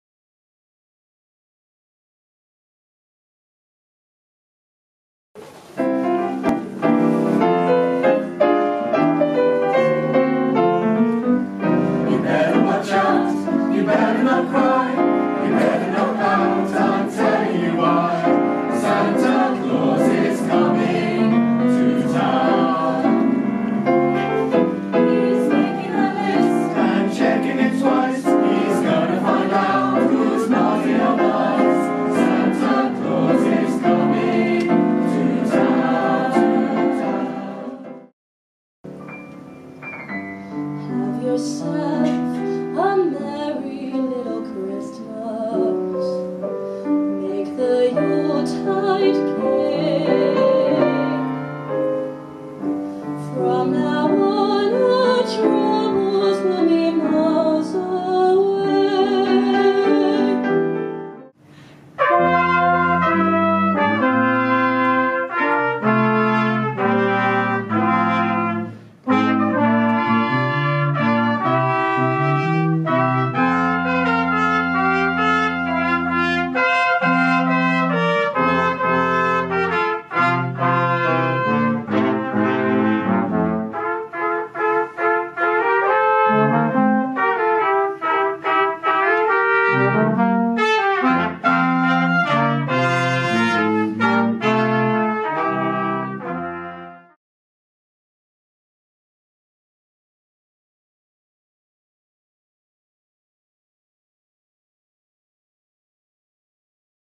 Christmas Concert 2017
Highlights from The Sixth Form College, Solihull's 2017 Christmas Concert, including festive songs from the College Choir and a Brass performance.